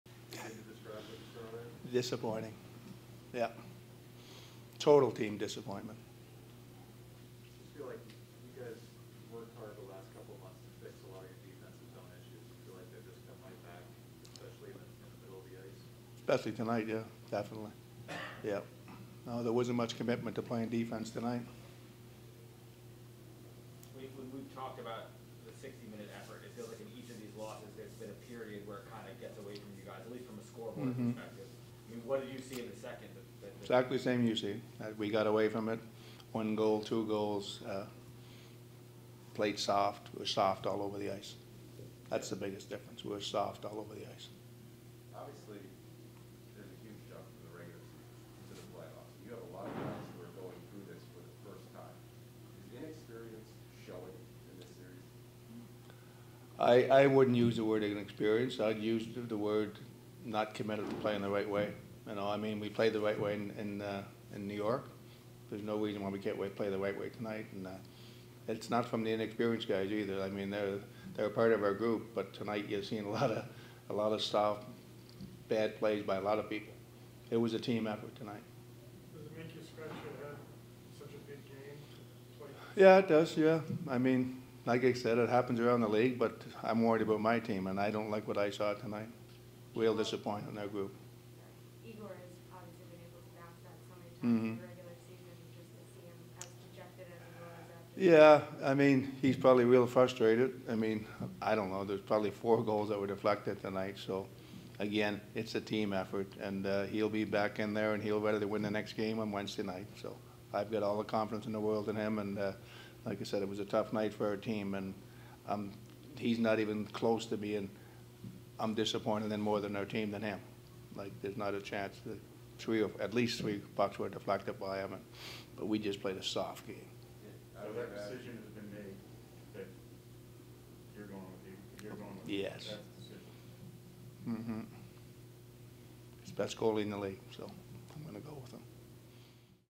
New York Rangers: Coach Gallant Postgame Media Availability | May 9, 2022
New York Head Coach Gerard Gallant answers questions from the media following the  May 9th game vs the Pittsburgh Penguins.